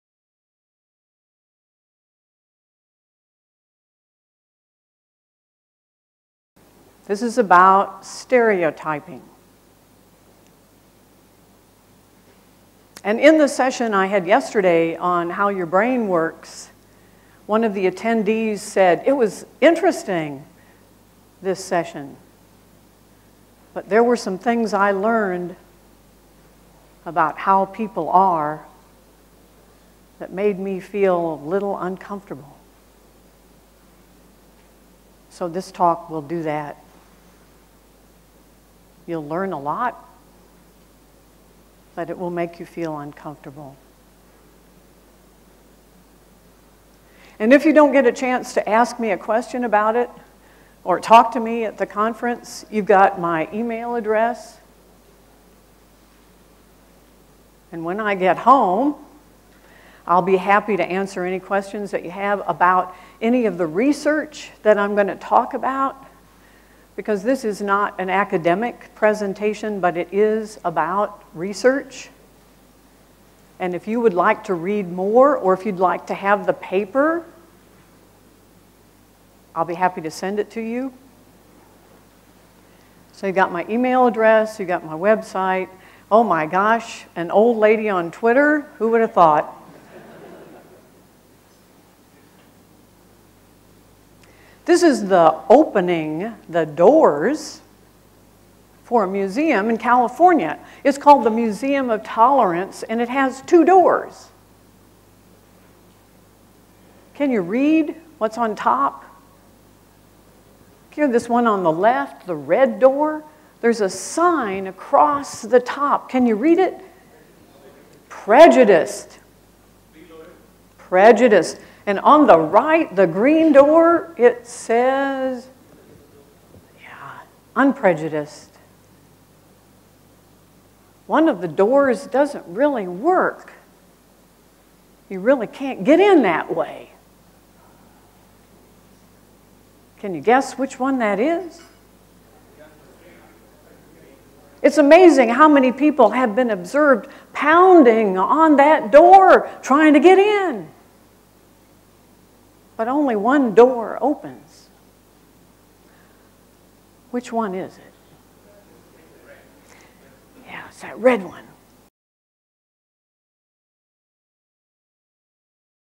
Who Do You Trust? Beware of Your Brain by Linda Rising
Cognitive scientists tell us that we are more productive and happier when our behavior matches our brain's hardwiring—when what we do and why we do it matches the way we have evolved to survive over tens of thousands of years. One problematic behavior humans have is that we are hardwired to instantly decide who we trust. And we generally aren't aware of these decisions—it just happens. Linda Rising explains that this hardwired "trust evaluation" can get in the way of working well with others. Pairing, the daily stand-up, and close communication with the customer and others outside the team go a long way to overcome our instant evaluation of others. As Linda helps you gain a better understanding of this mechanism in your behavior and what agile processes can do to help, you are more likely to build better interpersonal relationships and create successful products.

Speaker: Linda Rising (@RisingLinda)
Independent consultant, Linda Rising LLC

Conference: Agile India 2013 Conference